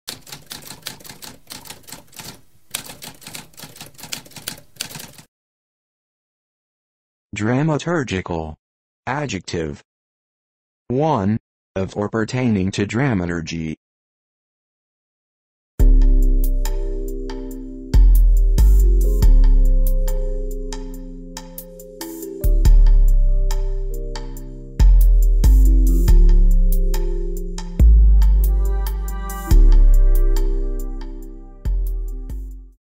What does dramaturgical mean?
A spoken definition of dramaturgical.

Intro Sound:
Typewriter - Tamskp